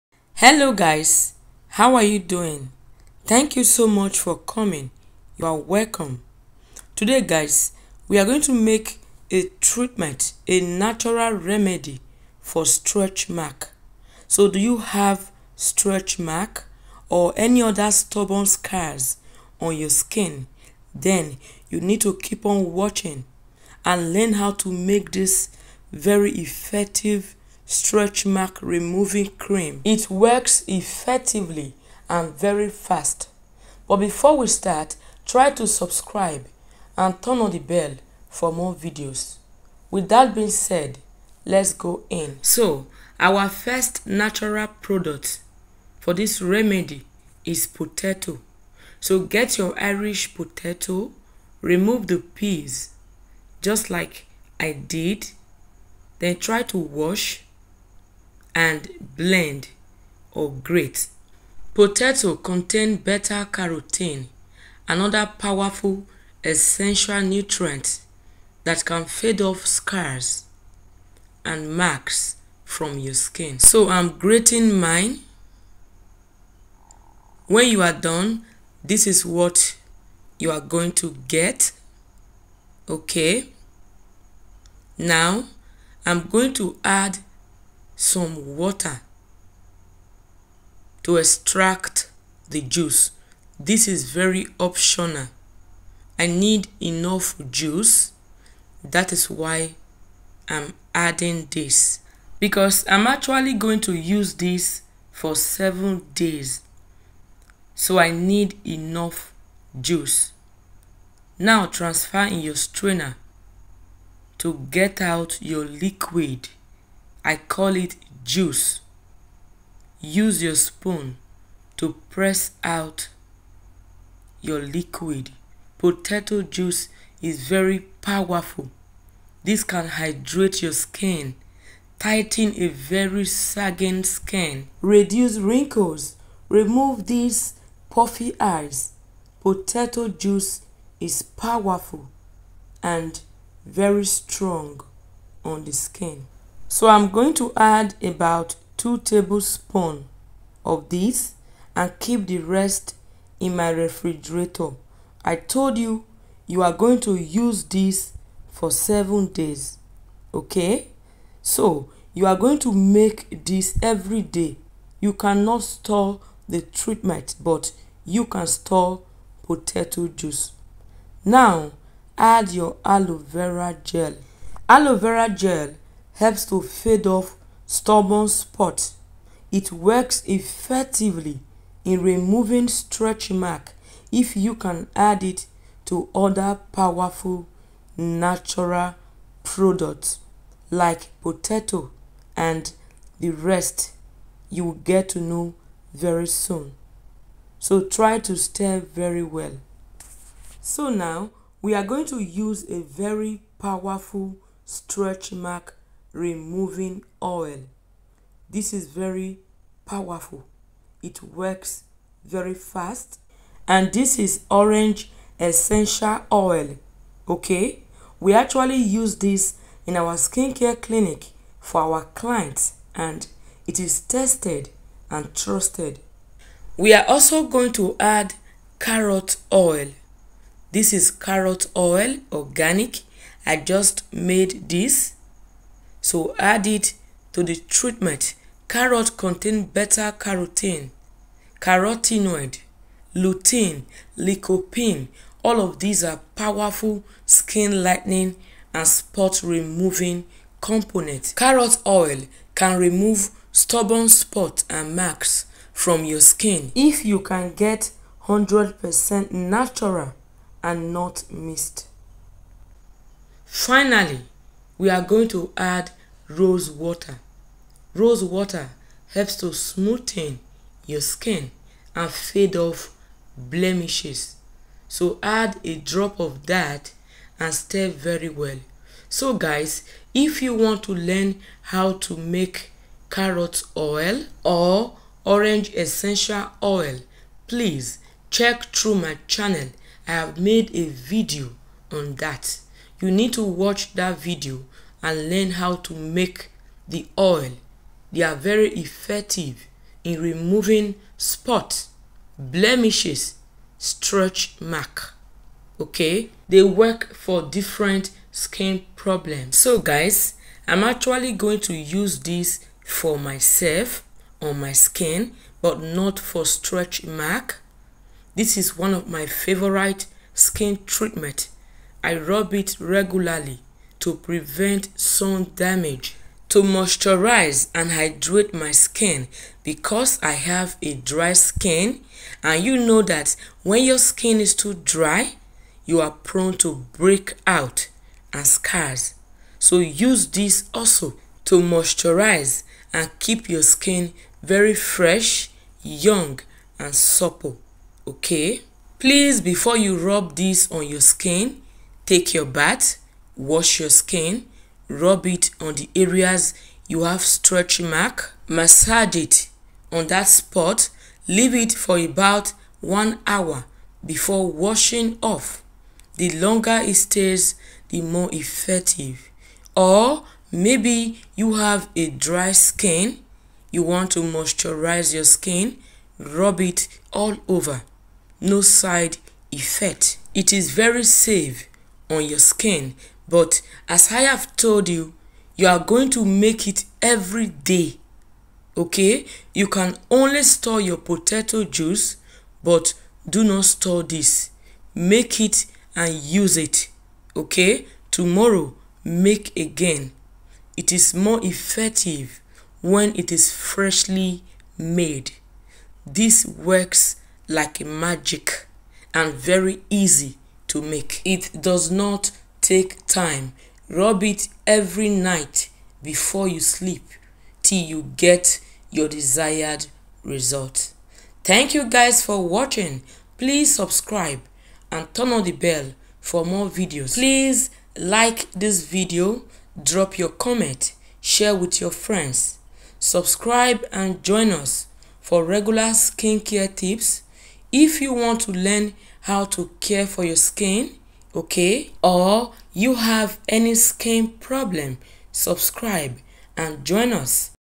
How to remove stretchmark with Potato | Remove Stretchmark at home in just 5 days
stretchmark natural remedy
Use this natural treatment to remove stretchmark permanently in 5 days,it is very effective and fast.

Potato is a powerful scars removing product,add few ingredient to potato and permanently remove stretchmark, remove stretchmark permanently.

Rub this every night on the affected area it really works, tested and trusted,100% active,so if you have been treating your stretchmark no result,try this and thank me later